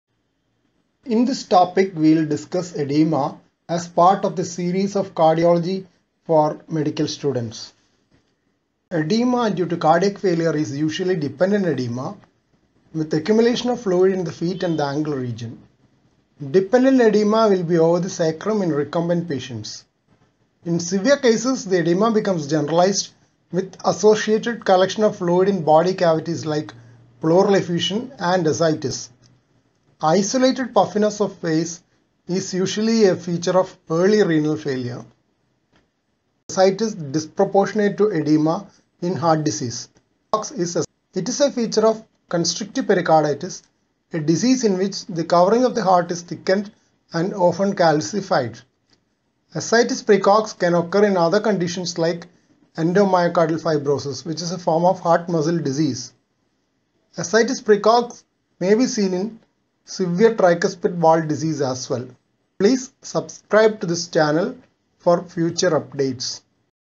Edema - Cardiology for Medical Students
Edema due to cardiac failure is usually dependent edema with accumulation of fluid in the feet and ankle region.

It will be over the sacrum in recumbent patients. In severe cases, the edema becomes generalized, with associated collection of fluid in body cavities like pleural effusion (collection of fluid in the pleural cavity) and ascites (collection of fluid in the peritoneal cavity). Isolated puffiness of face is usually a feature of edema of early renal failure.